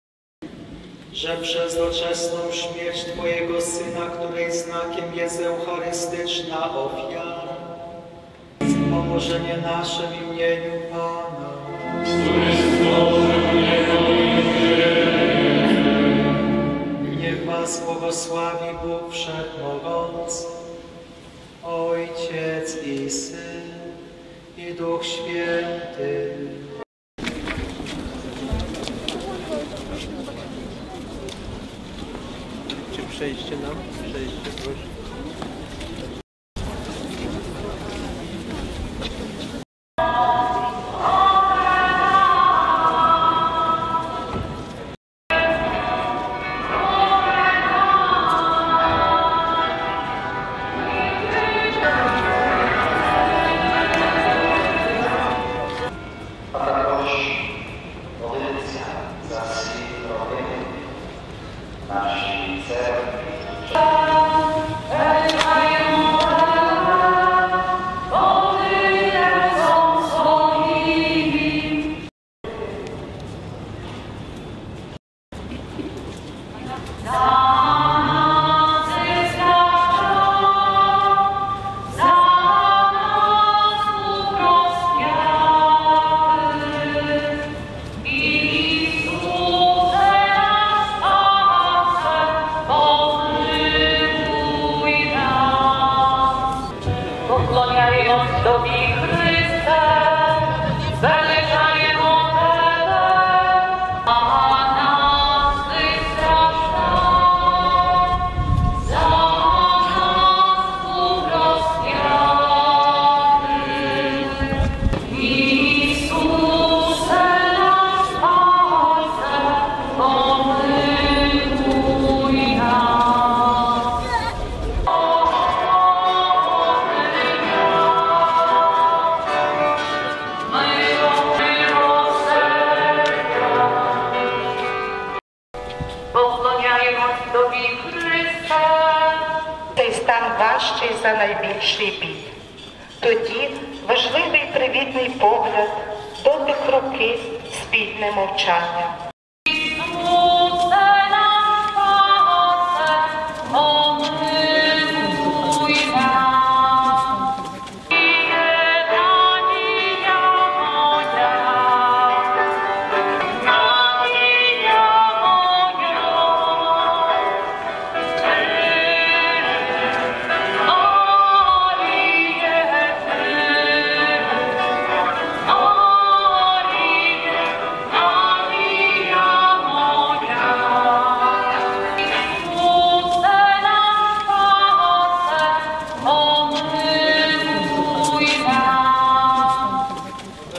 Хресна Дорога у Львові
Увечері 17 березня, у Велику Середу, від Митрополичої Базиліки до санктуарію св. Антонія у Львові вулицями міста пройшла Хресна Дорога римо-католиків, яку традиційно організовавують отці францисканці конвентуальні.

Вірні Львівських парафій римсько-католицької Церкви об`єдналися, щоб разом роздумувати над стражданнями Господа Ісуса Христа та просити Його про милосердя для нашої держави, для нашого міста та родин. Благали Бога про припинення війни у нашій Батьківщині та про мир у власних серцях.

Хресну Дорогу очолив Архієпископ Мечислав Мокшицький. Разом з ним були єпископ-помічник Едуард Кава, священики, монахині, семінаристи, вірні місцевих парафій.